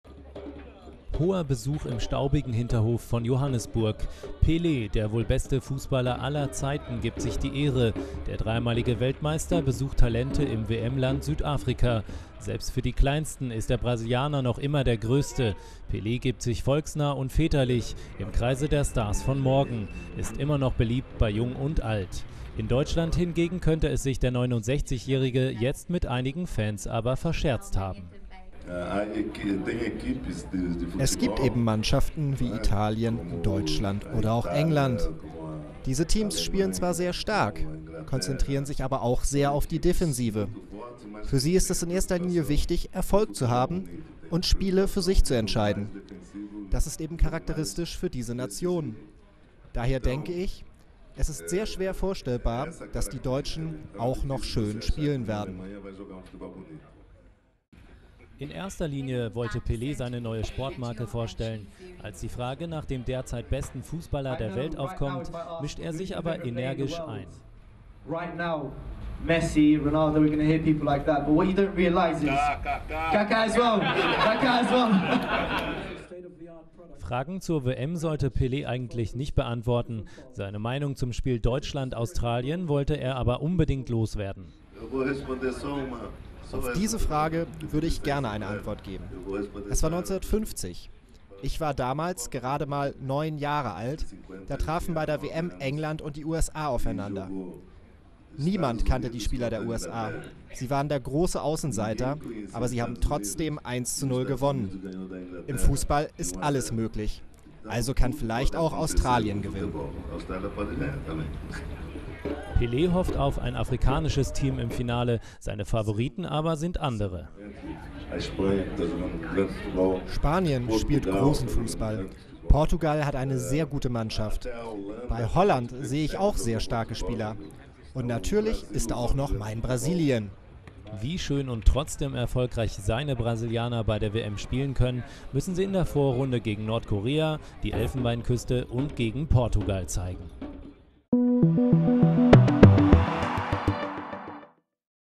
Pele: "Deutschland kann nicht schön spielen"
Pele: "Deutschland kann nicht schön spielen"Der vielleicht größte Fußballer aller Zeiten hat in Johannesburg seine Sportmarke präsentiert. Und dabei einen kräftigen Seitenhieb auf den deutschen Fußball ausgeteilt.